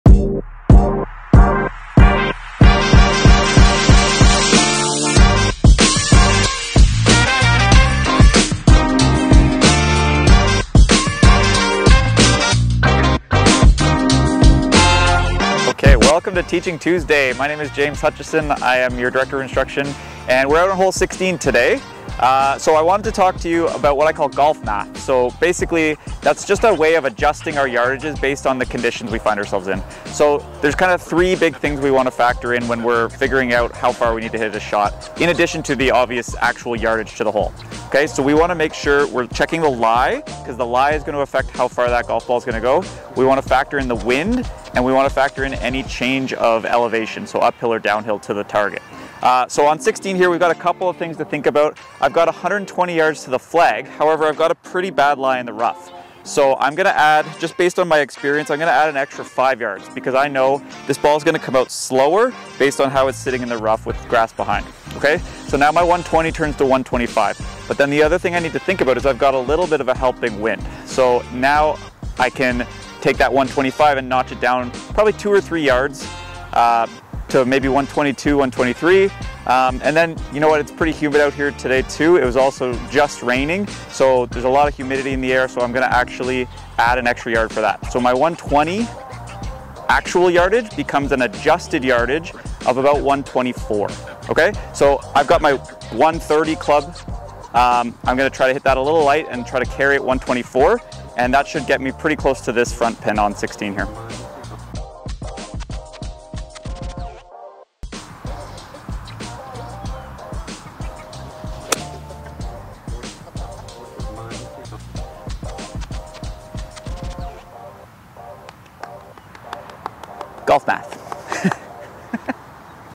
GOLFMATH! - Teaching Tuesday
James explains a technique he calls "Golf Math" and how to properly adjust the yardage to outside conditions to get better aimed shots.